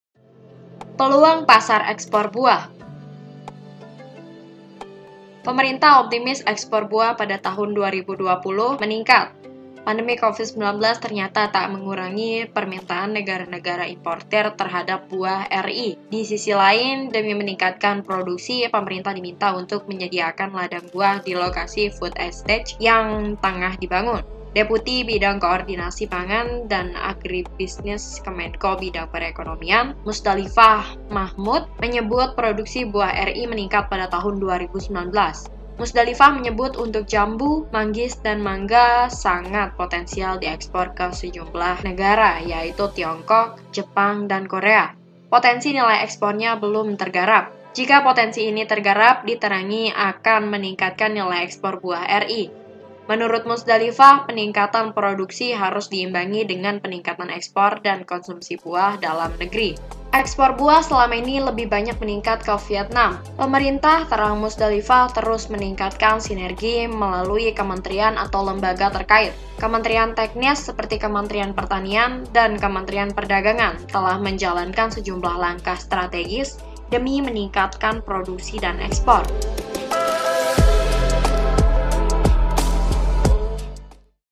Peluang Pasar Ekspor Buah
Pemerintah optimistis ekspor buah pada tahun 2020 meningkat. Pandemi Covid-19 ternyata tak mengurangi permintaan Negara-negara importir terhadap buah RI. Di sisi lain, demi meningkatkan produksi, Pemerintah diminta untuk menyediakan ladang buah di lokasi Food Estate yang tengah dibangun.